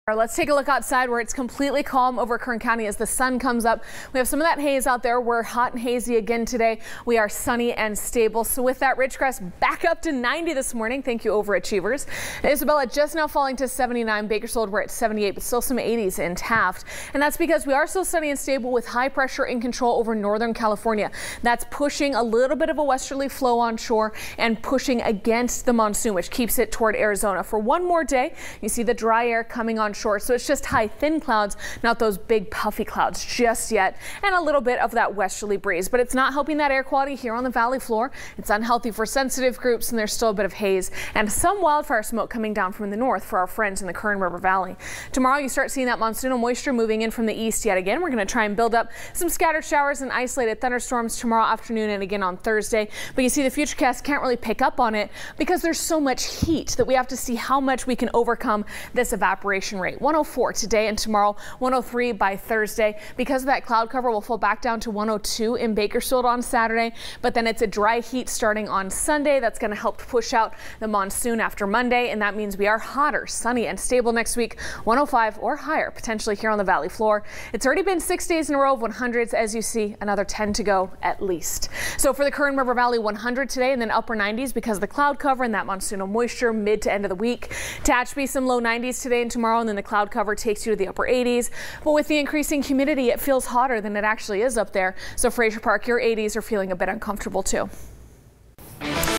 Storm Shield Forecast morning update 7/17/18
Today is Day #7 of this current heatwave with an expected high of 104 in Bakersfield and air quality that is once again unhealthy for sensitive groups. Up in the Kern River Valley, Lake Isabella and Kernville look to top out at that 100 degree mark again today with the low 90s in Tehachapi and the upper 80s in Frazier Park. At least it's a dry heat for now!